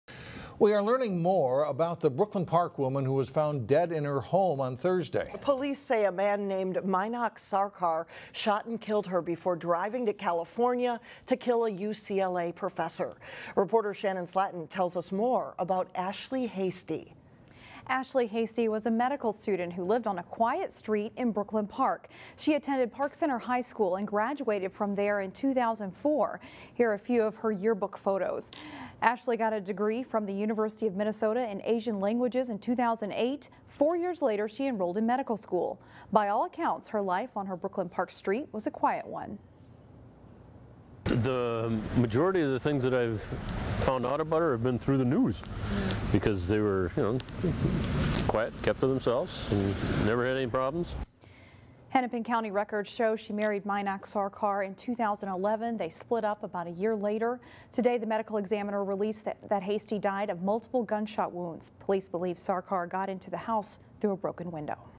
Brooklyn Park shooting victim was medical school student
People around the nation heard about Ashley Hasti, the woman who was allegedly murdered by her estranged husband before he drove to UCLA, shot a professor there, and then turned the gun on himself. Her name made headlines on Thursday as a victim on Mainak Sarkar's kill list in a very bizarre tragedy.
But who was the woman who lived quietly on Pearson Parkway in Brooklyn Park?
Neighbors say she kept to herself. "The majority of things I've found out about her have been through the news because they were quiet and kept to themselves. We never had any problems," says Gordy Aune, Jr, the neighborhood watch block captain who lived only a few doors away from Hasti. Records show her father owned the property and neighbors say he lived there at times.
Records also show Ashley Hasti was a good student. She graduated from Park Center High School in 2004 before going to the University of Minnesota and churning out a degree in Asian languages by 2008. Hasti also took classes at North Hennepin Community College and worked as a tutor there. She spent time in California, where she met her husband, before starting medical school at the University of Minnesota in 2012. Hasti's Facebook timeline is full of posts about completing papers and studying for exams.
Her Facebook feed also reveals she tried stand-up, improvisational comedy. It shows photos and posts with her family and friends. It does not have much about her estranged husband, who records show she married in 2011.
Police have not released a motive in the killing.